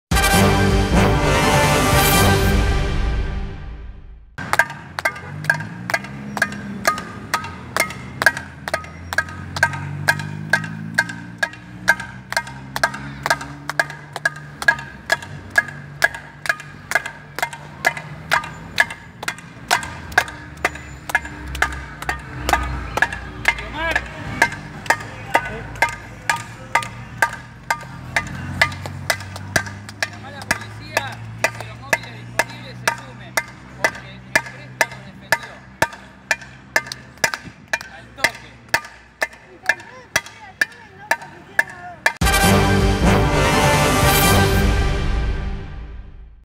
Cacerolazo en Campana por la detención de un “twitero” que amenazó a Cristina Fernández
Se trata de un comunicador de las redes conocido como “El Presto”, quien fue denunciado por un abogado luego de escribirle en su cuenta de Twitter a la ex Presidenta "Vos no vas a salir viva". En la escalinata de la Plaza Eduardo Costa, tibio reclamo en “defensa de la libertad de expresión”.